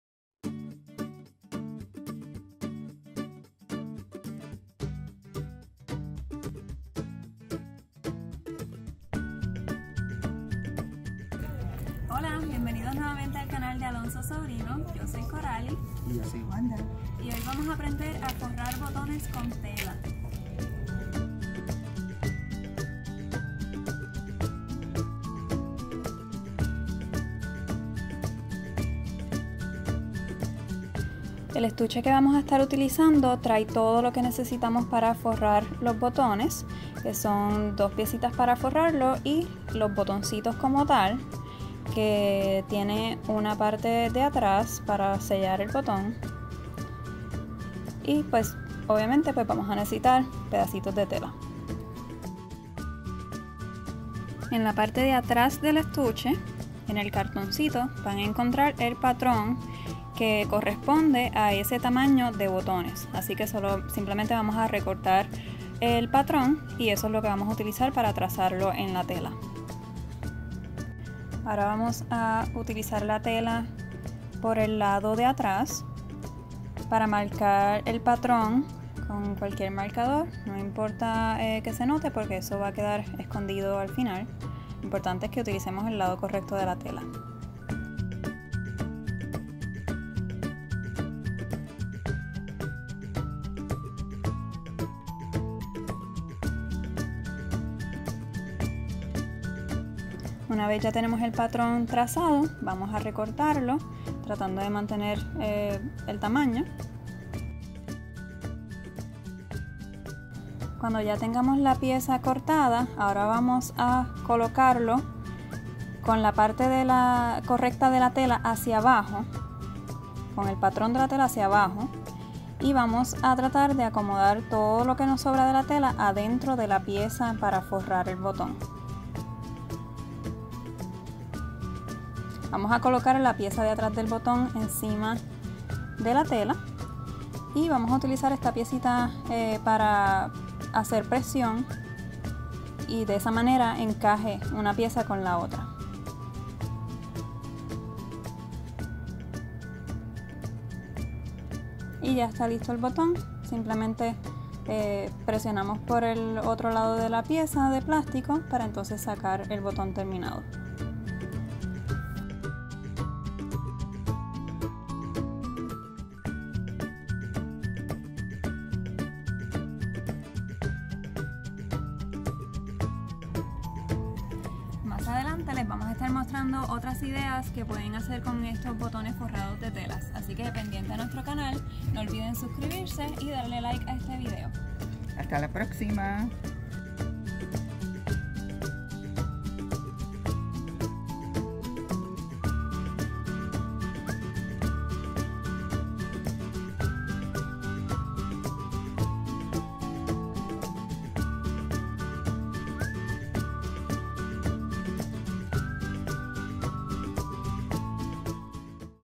DIY • Fabric Covered Buttons
Making fabric covered buttons is so easy! By using this kit, you can make your buttons in just a few simple steps. These can be used for your clothes, cushions, and even earrings! Have fun and get creative with it ♥

MATERIALS:

• Cover Button Kit
• Scissors
• Fabric

Everything you need to make this project can be found in Alonso Sobrino! ♥